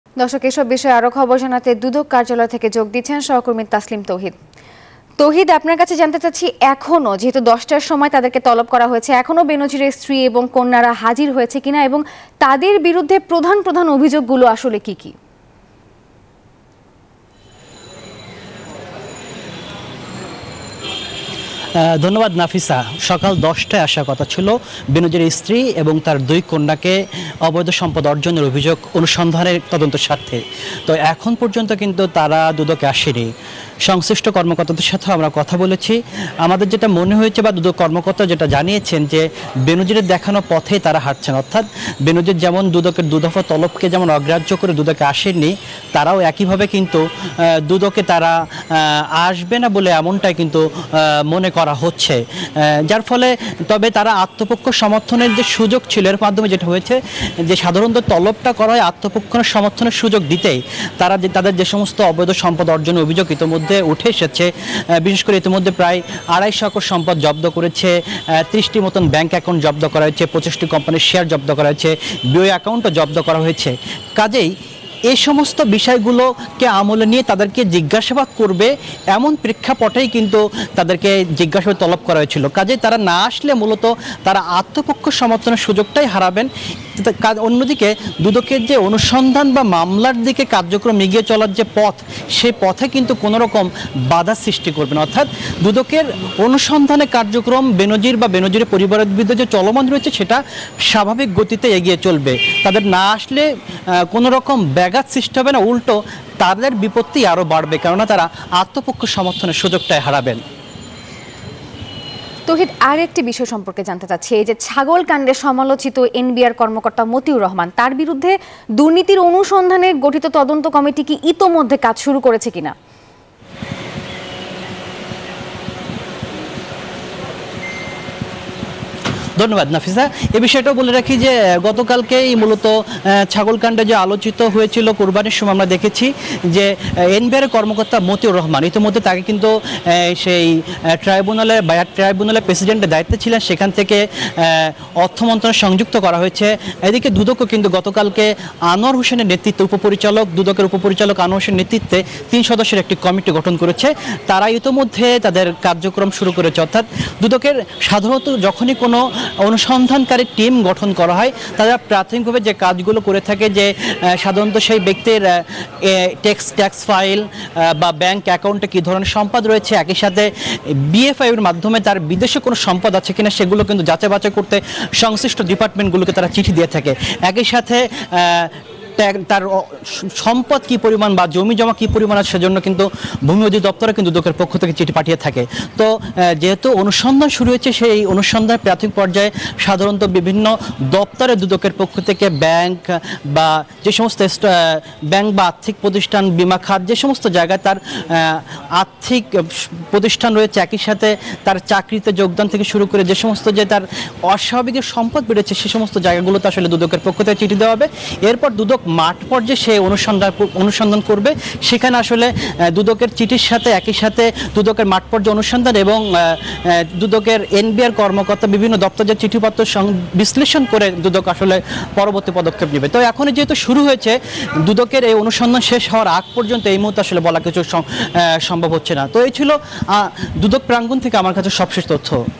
বেনজীরের স্ত্রী-কন্যাদের বিরুদ্ধে প্রধান প্রধান অভিযোগগুলো কি? | News24
NEWS24 Team